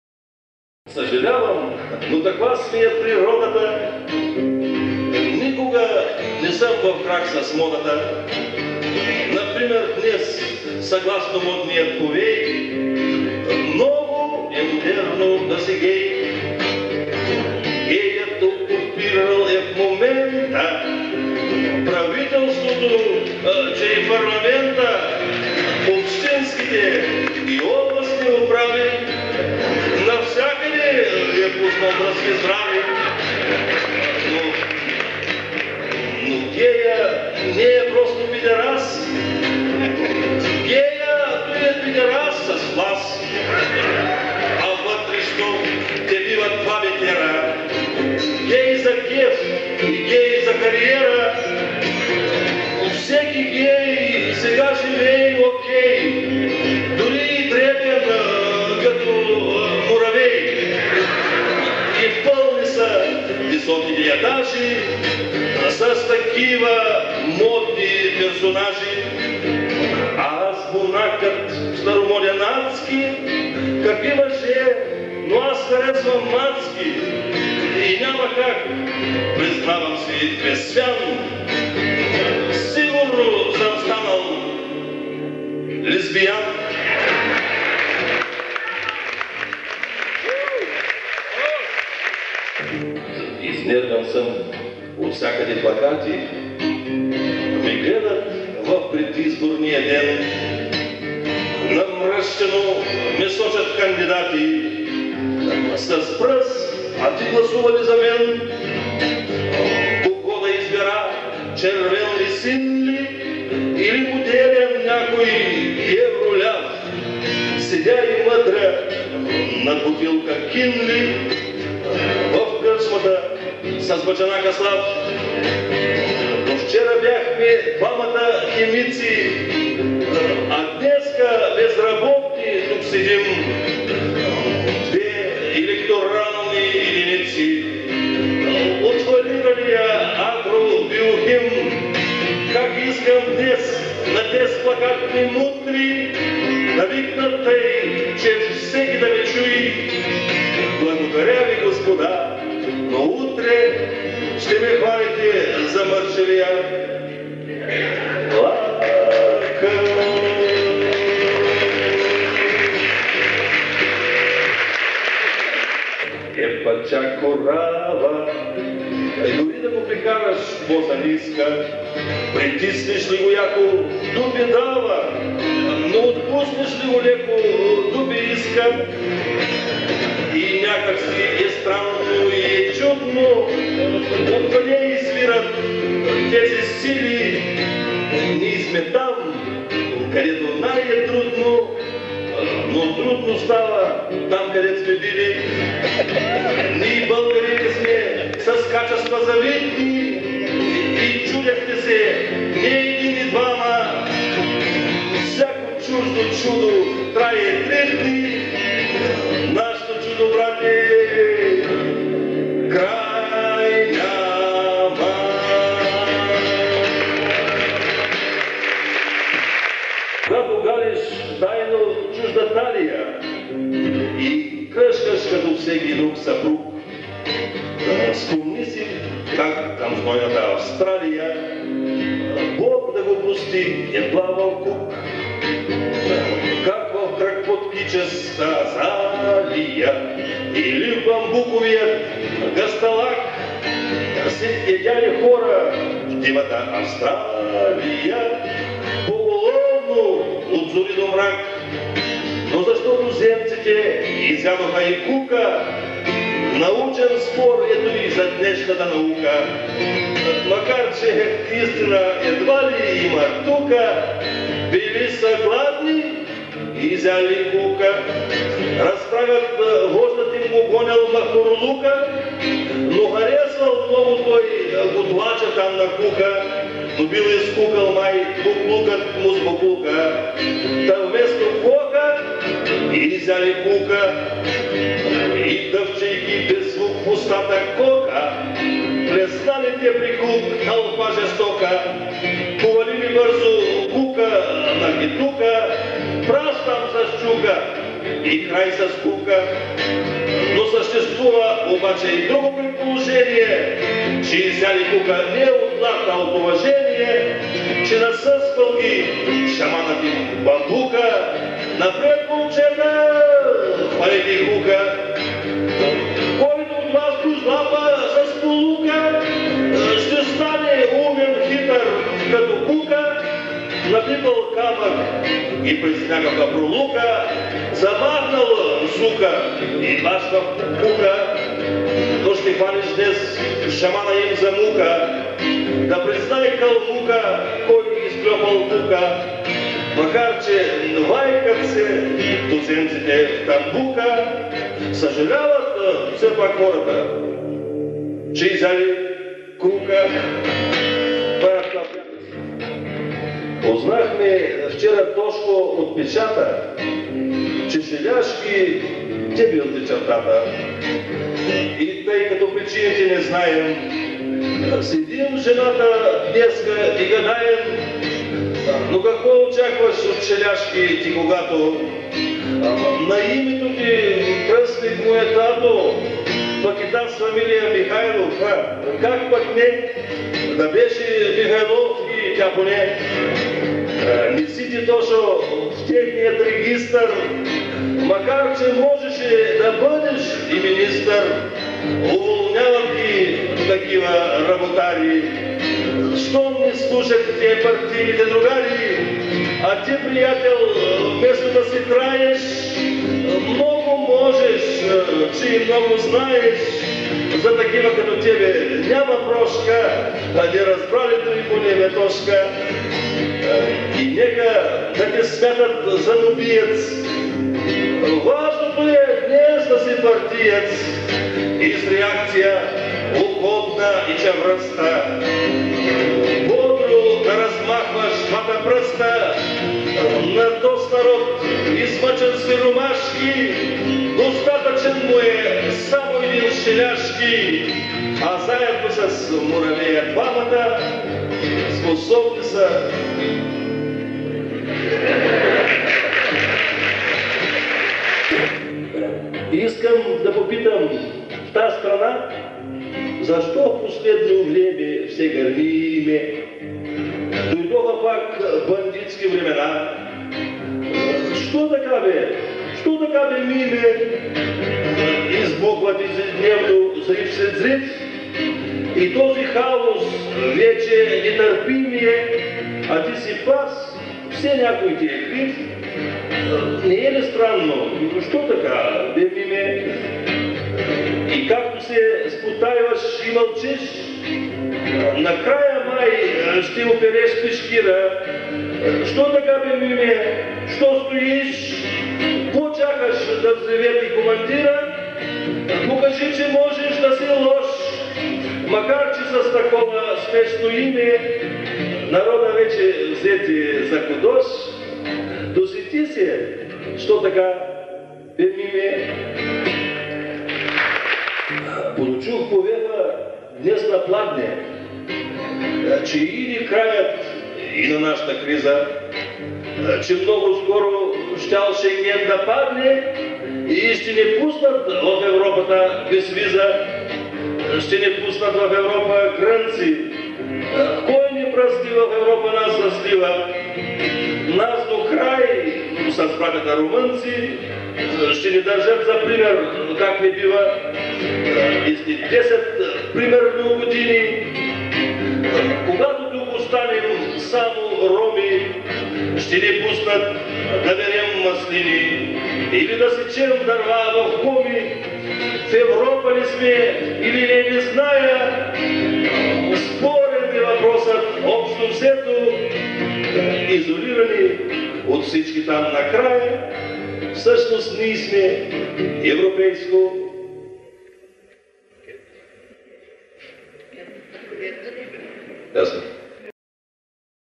Поетът с фабрика - първи официален концерт
Усмивки от старите ленти: Концерт в залата на старозагорския театър по случай 50-годишнината на Митко Динев - поета с фабрика (фабрики).